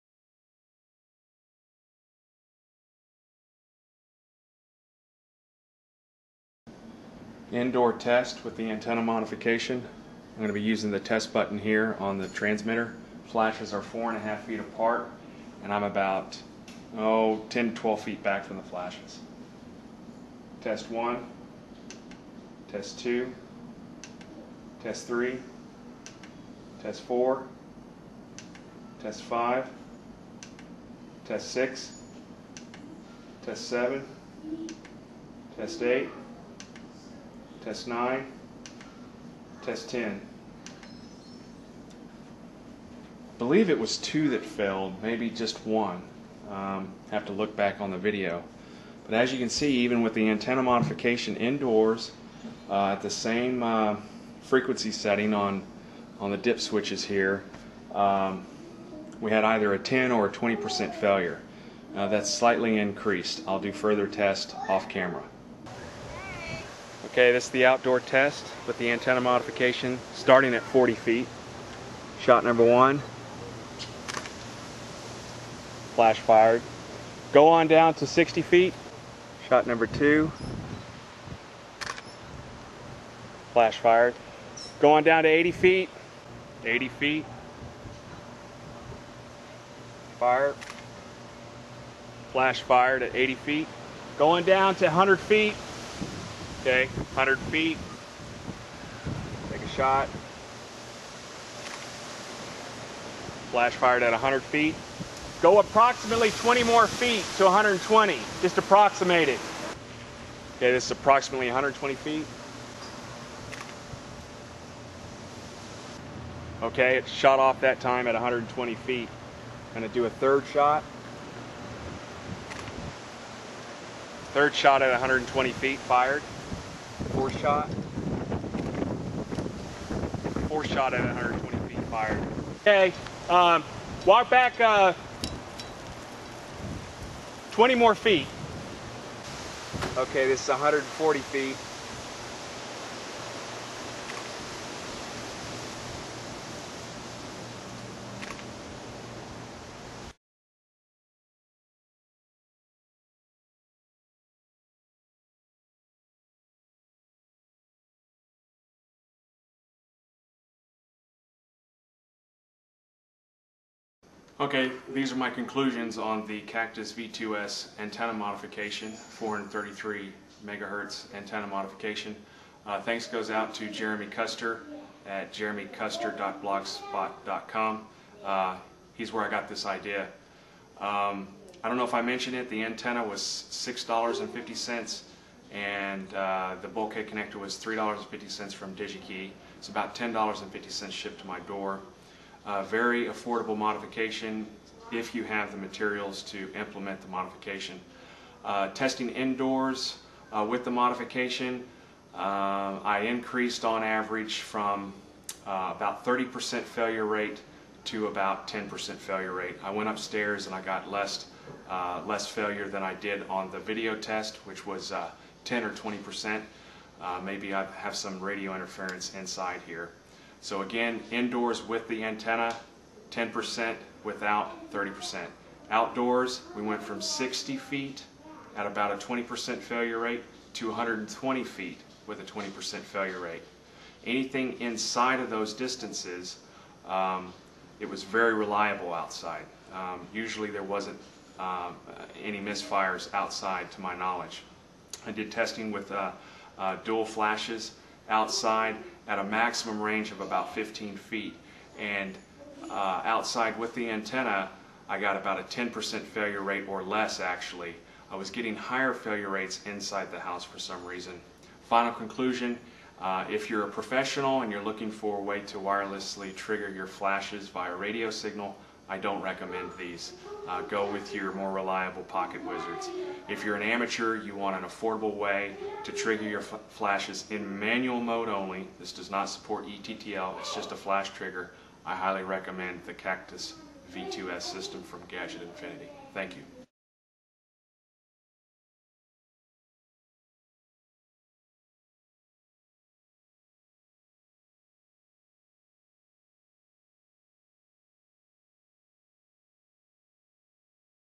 Cactus V2s Wireless Flash Trigger Review 4 of 4
Video 4 of 4 detailing my experience with the Cactus V2s wireless flash trigger system.  Includes two modifications to the flash trigger system.